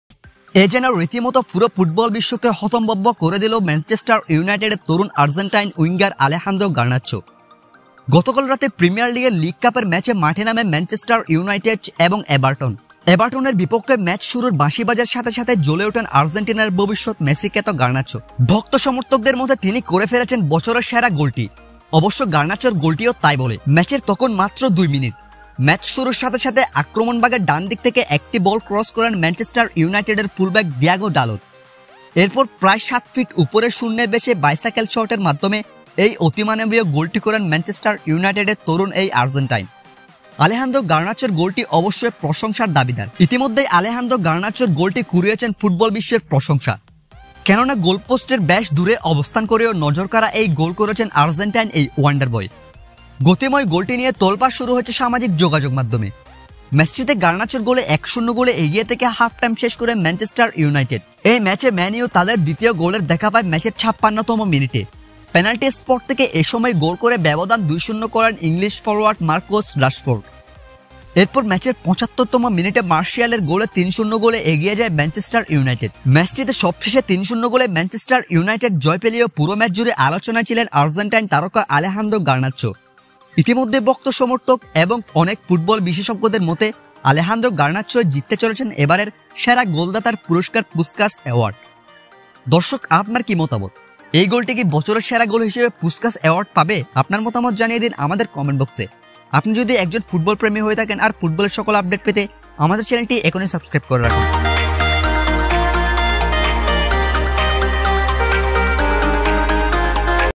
একি দেখলাম 😱 ৭ ফুট ওপরে ভেসে আর্জেন্টাইন তারকার জাদুকরী গোল❗বছরের সেরা গোলের পুরস্কার পেল এবার
Gernacho Bicycle kick goal Manchester United